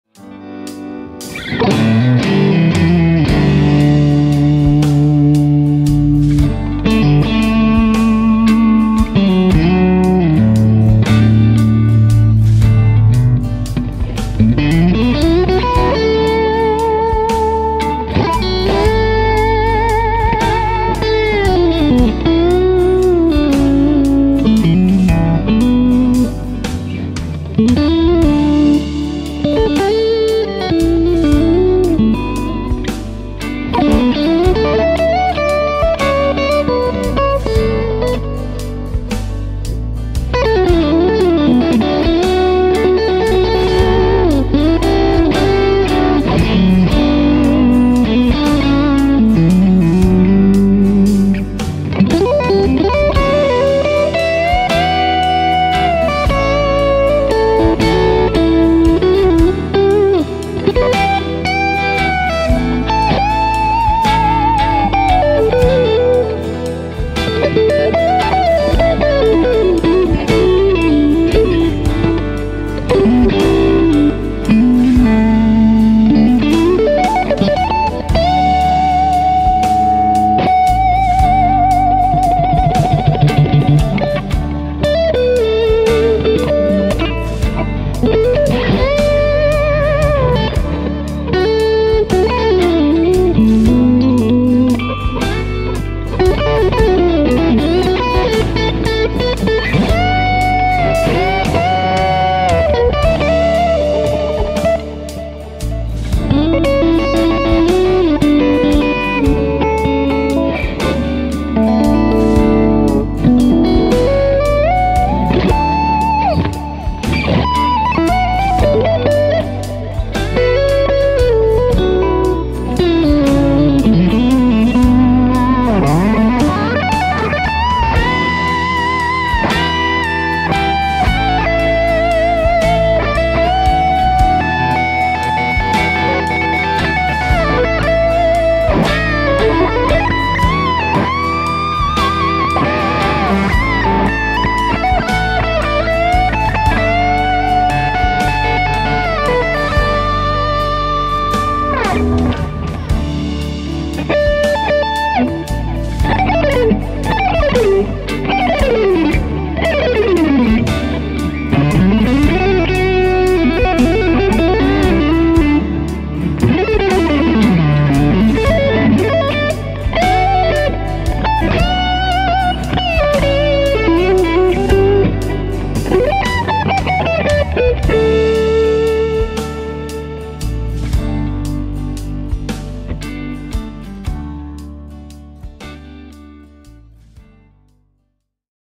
Sunday Morning Guitar with Love
Testing Sounds of my new Schecter Sunset  Custom Shop HSS